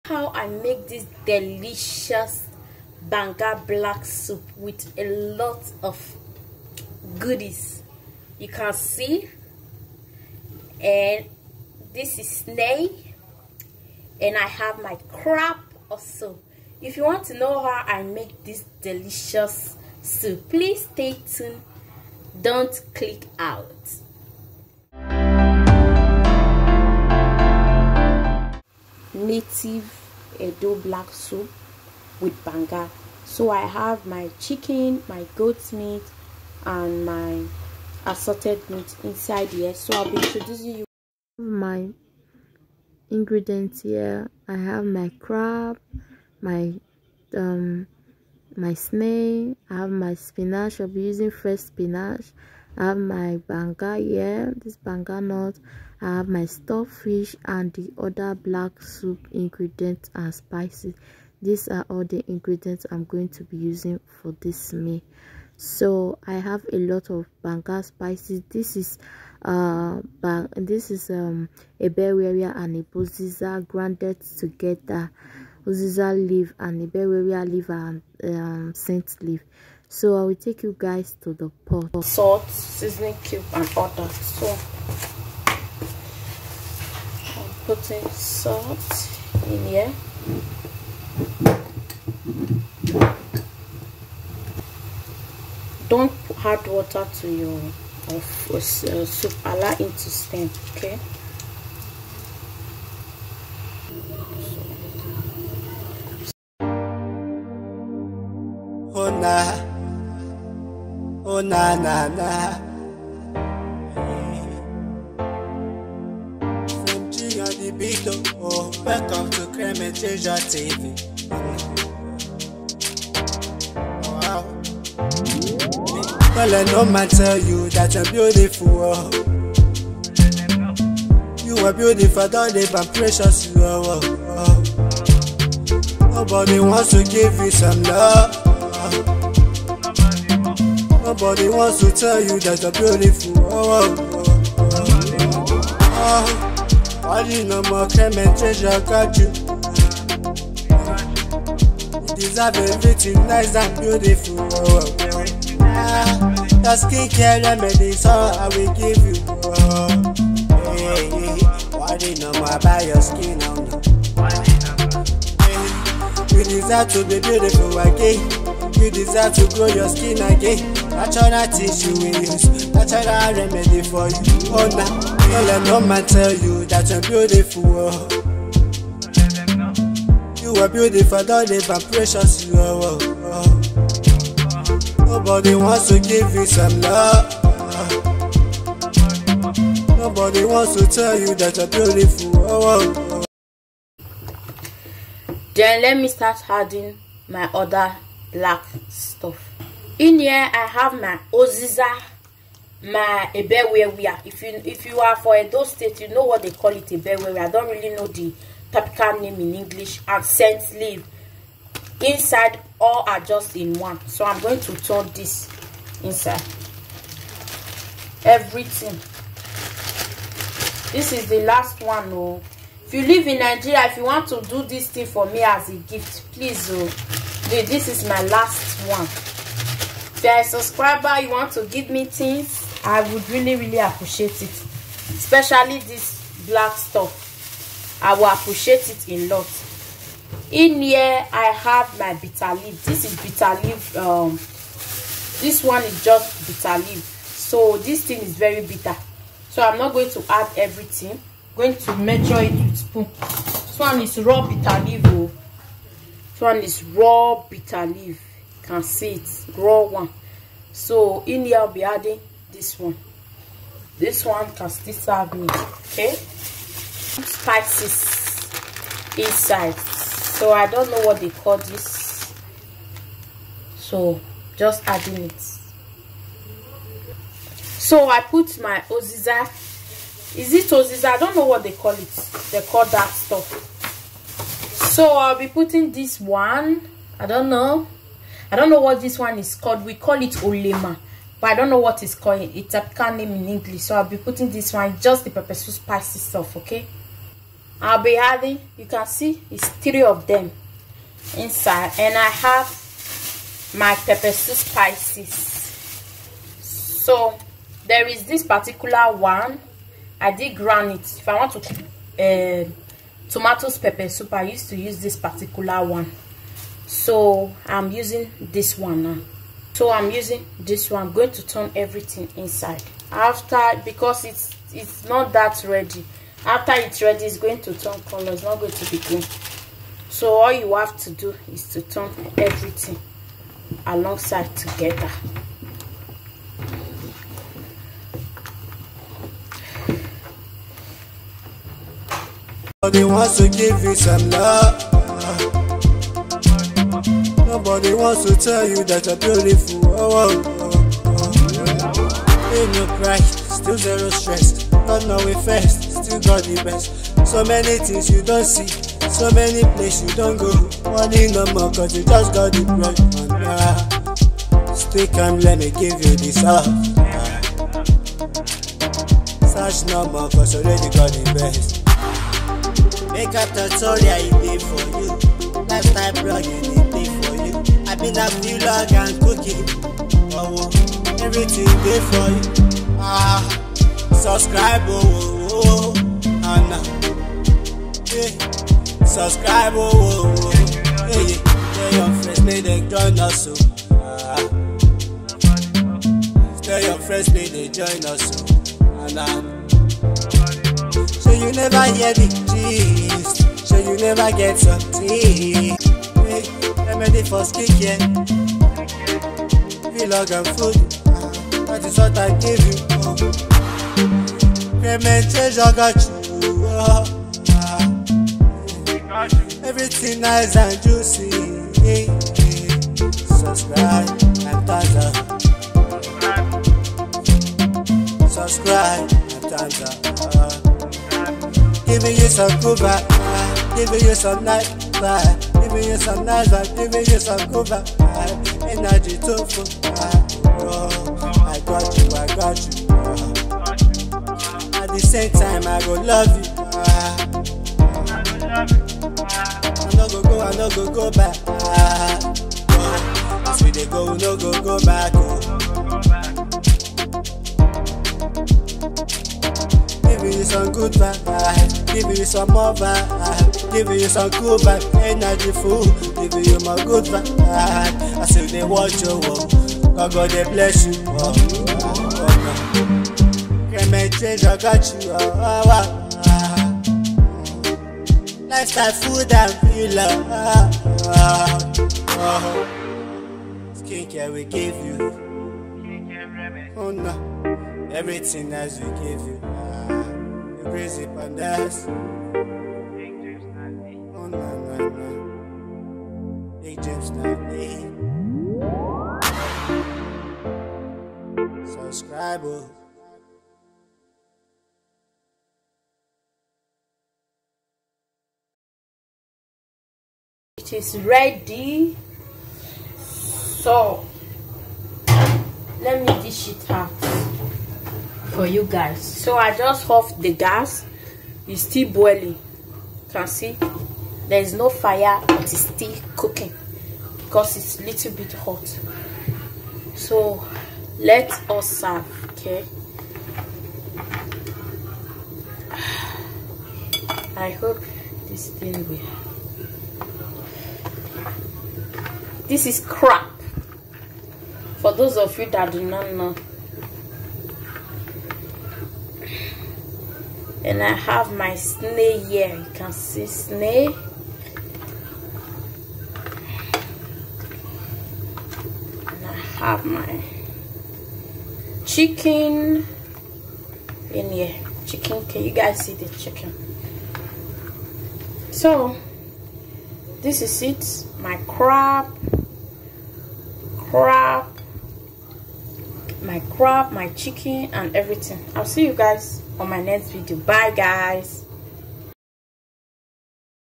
how to cook native black soup with Banga. how to make traditional Edo black soup with bitterleaf
ingredients on this soup
spinach leaf  bitterleaf powder
Black powder ozizi leaf
pepper soup spice
goat meat  crabs more